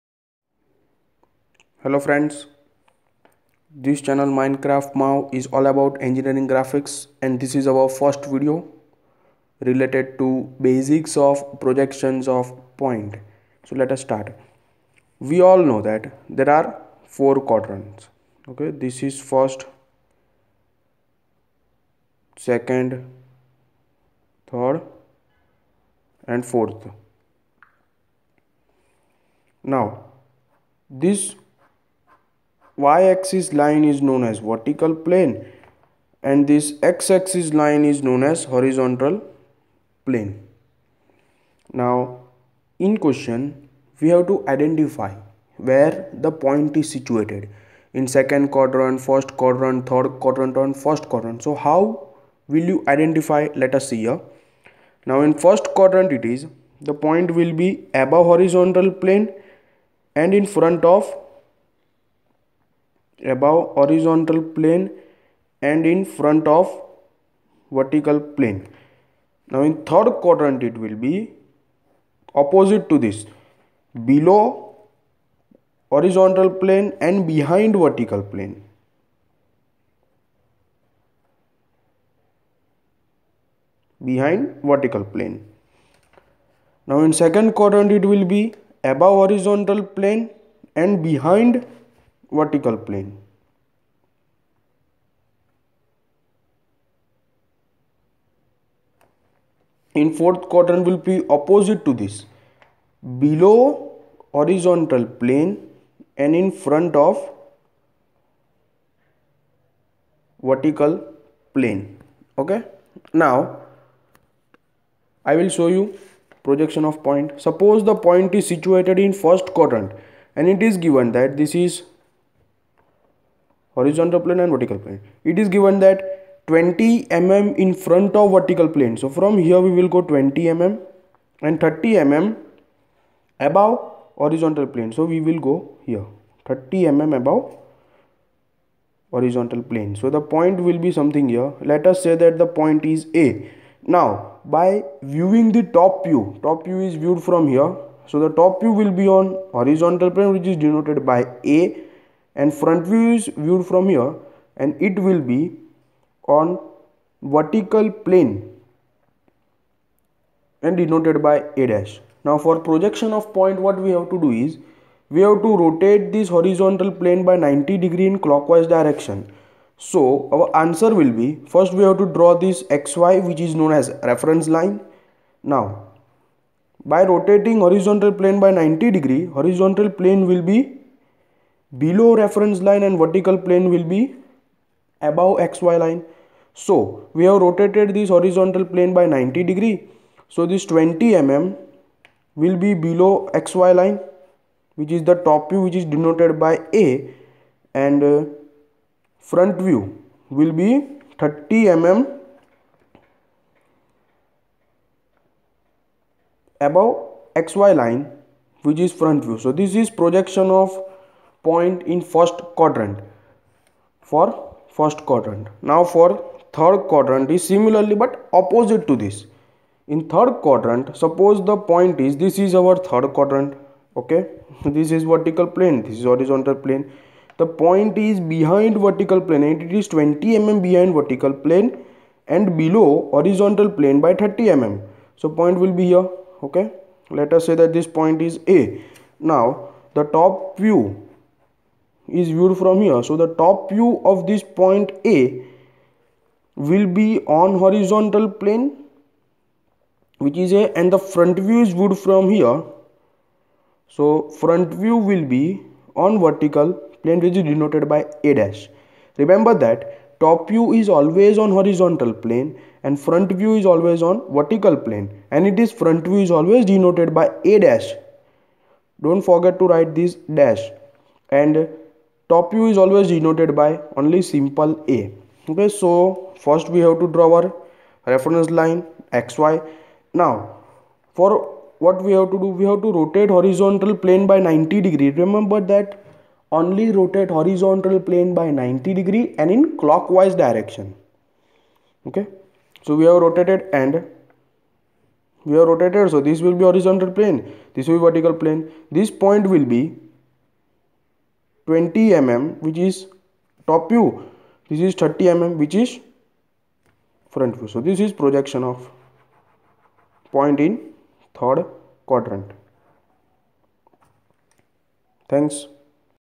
Projection of points
Get an idea of projections of point, top view and front view in 1st and 3rd quadrant.